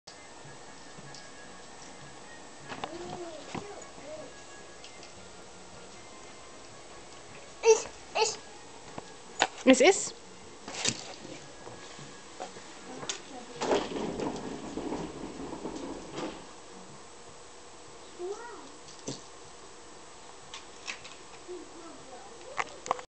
Now, that's more like it!
Gideon tries my poi